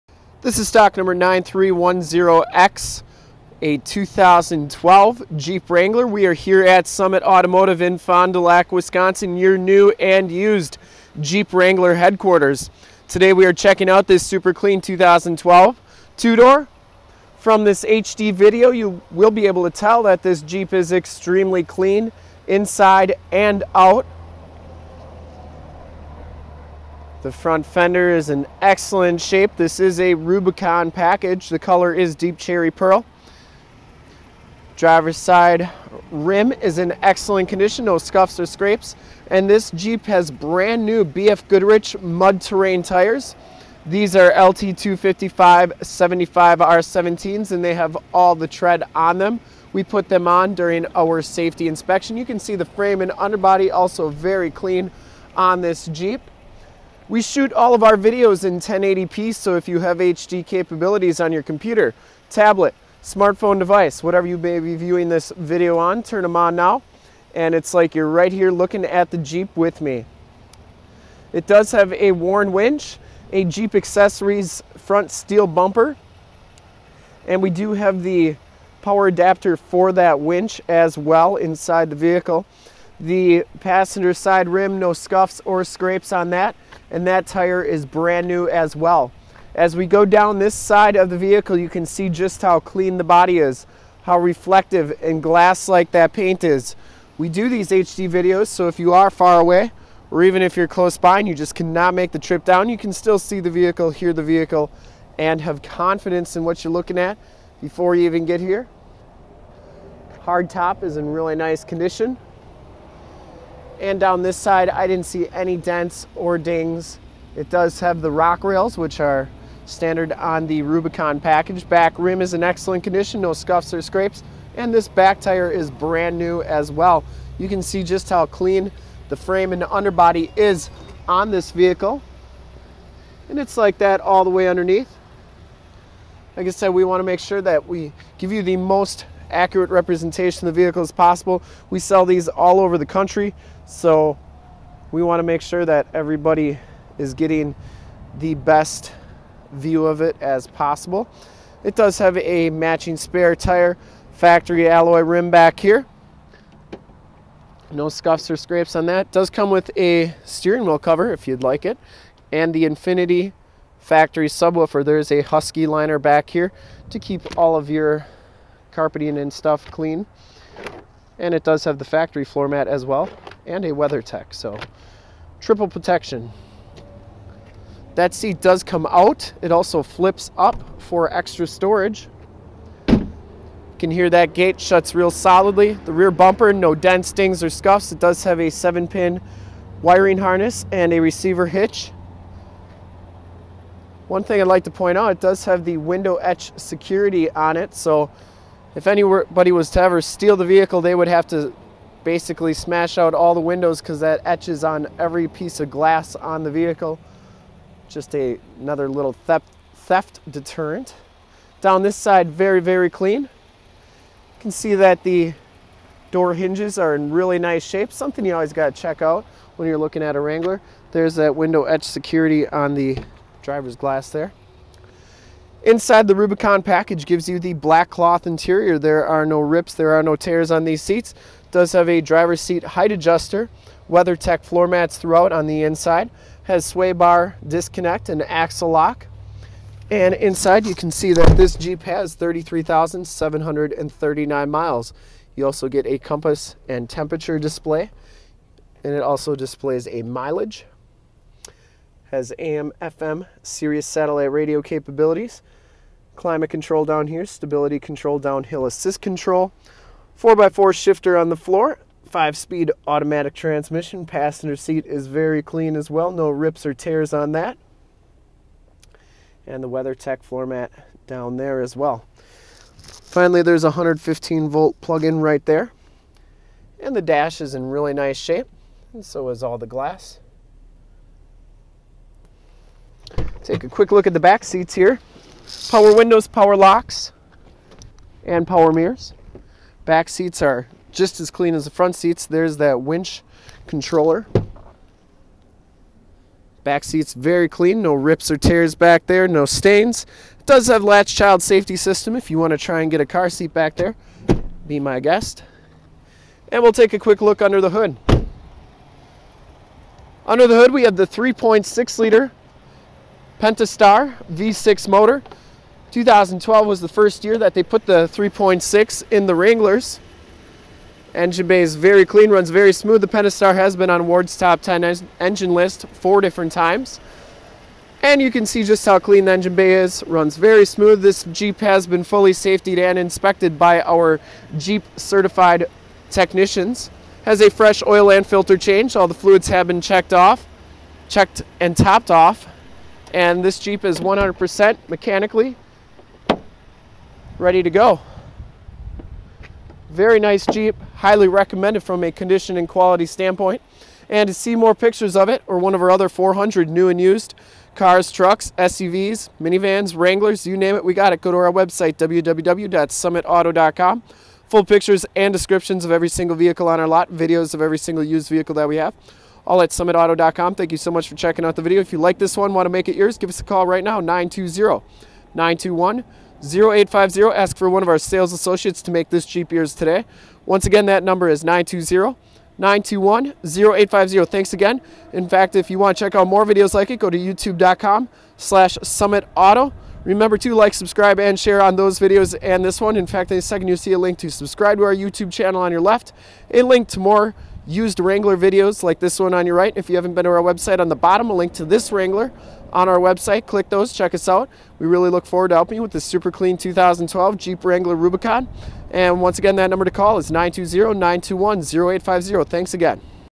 2012 USED JEEP WRANGLER RUBICON DEEP CHERRY WALK AROUND 9310X SOLD!!
STOCK: 9310X
PRICE: $27,499
MILES: 33,733
MAKE: JEEP
MODEL: WRANGLER RUBICON
VIN: 1C4BJWCG3CL101926

3.6 Liter V6 DOHC Engine, 285 Horsepower, Two Door with 3-Piece Modular Hard Top, Rubicon Package, Automatic Transmission Center Console Shifter, 5 Speed Automatic Transmission with Optional Manual Tap Shift, 4x4 Floor Shifter Four Wheel Drive 4WD, Driver Seat Height Adjuster, Non Smoker, Black Ebony Cloth Seats, Bucket Seats, 2nd Row Bench Seating, Towing Package with Receiver Trailer Hitch and Trailer Wiring Tow Package, Heated Power Mirrors, Electronic Stability Control Traction Control ESC, Axle Lock, Downhill Assist Control DAC, Sway Bar Link Disconnect, 3.73 Gears, BF Goodrich Mud Terrain T/A LT255/75 R17 Tires, Factory Alloy Rims Premium Wheels, Four Wheel Disc Brakes, Fog Lights, AM / FM Radio Tuner, Sirius/XM Satellite Radio Capabilities Sirius / XM, CD Player, Alpine Premium Audio Sound System, Auxiliary MP3 Jack Portable Audio Connection, Factory Subwoofer, Keyless Entry System, Rear Window Defroster, Swing Open Rear Door with Manual Raise Glass, Adjustable Height Seatbelts, Driver and Passenger Front Air Bags, L.A.T.C.H. Child Safety System, Multi-Function Steering Wheel Controls, Compass, Outside Temperature Display and Mileage Display, Weathertech Floormats, Air Conditioning AC, Cruise Control, Power Locks, Power Windows, Tilt Steering Wheel, Automatic Headlights Autolamp, 115V / 150W Auxiliary Power Outlet, Maroon,

STOCK: 9310X
PRICE: $27,499
MILES: 33,733
MAKE: JEEP
MODEL: WRANGLER RUBICON
VIN: 1C4BJWCG3CL101926